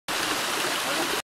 Como bajar una montania sin morir - Aconsejamos Lago San Martin - Santa Cruz - Argentina
Lago San Martin - Santa Cruz - Argentina
Best country
si